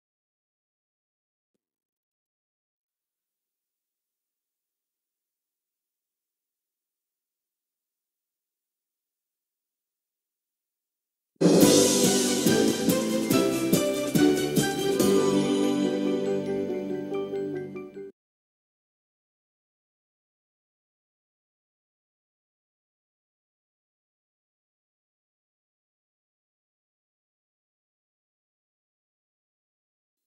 Opening To A Flea Market Documentary 2006 DVD
Here Is The Opening To A Flea Market Documentary 2006 DVD And Here Are The Order:
1.Navy Blue FBI Warning Screen
2.PBS Logo
3.DVD Menu
That's All.